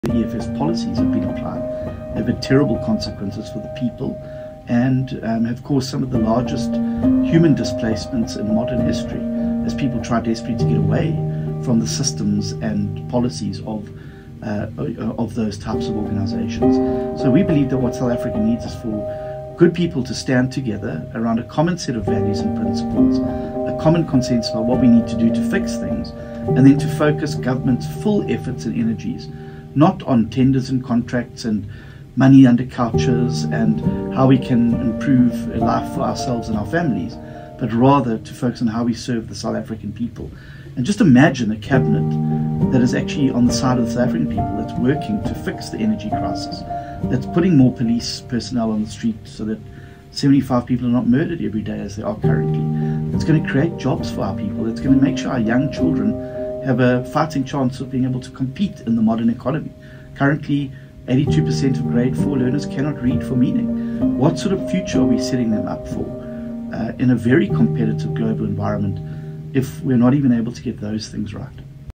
Vote DA to stop the EFF and rescue South Africa
The DA believes that South Africa needs good people to stand together around a common set of values and principles and focus government’s full efforts, not on tenders and contracts, money under couches and self-serving politicians, but rather on how we serve the South African people. 🇿🇦